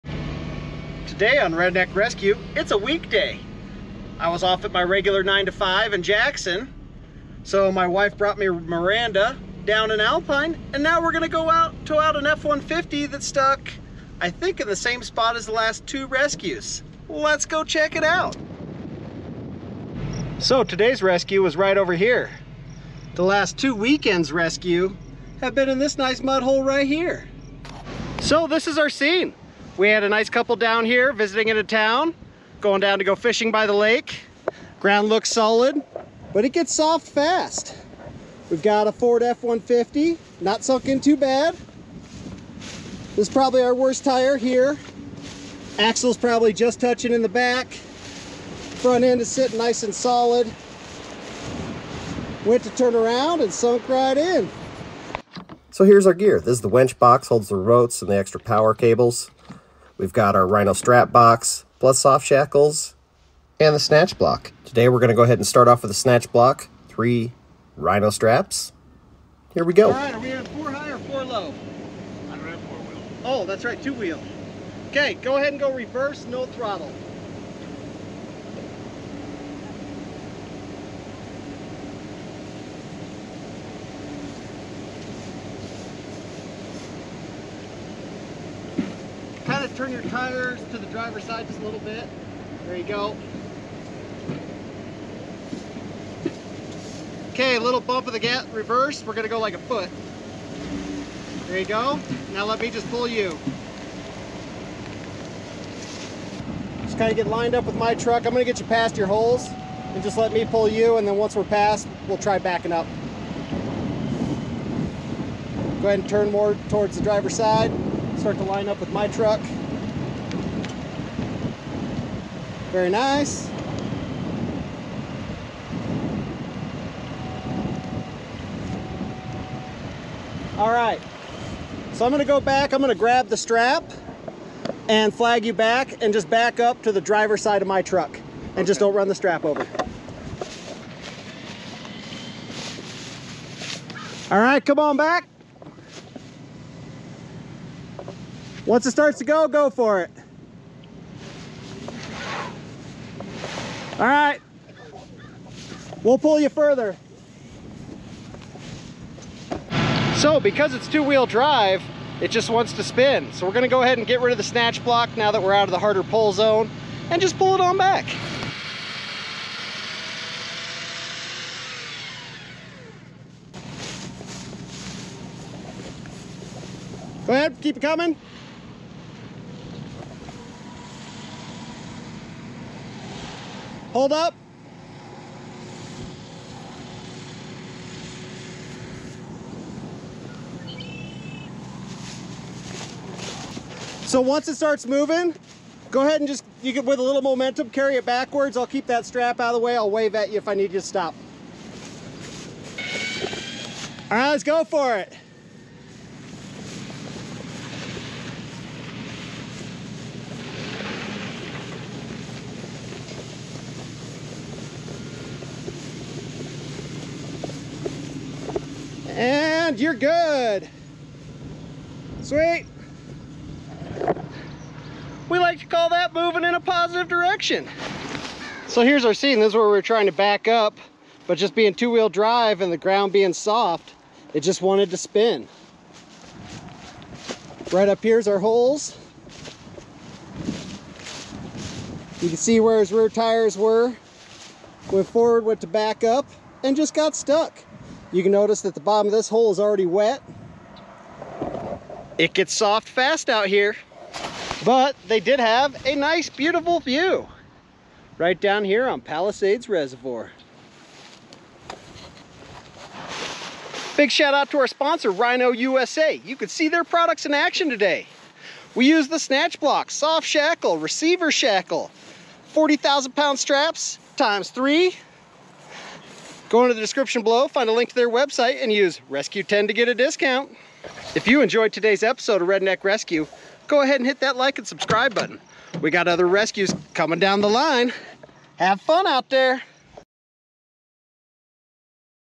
Strike Three In The Rut Hole!!offroadrecovery
Today on Redneck rescue our situation is we’ve got a Ford F150 stuck down in Palisades Lakebottom. We’ve been to the same spot three rescues. It was a nice couple visiting. They got stuck in their 2 Wheel Drive pick up. Or taking our rhino gear and Miranda to go pull them out.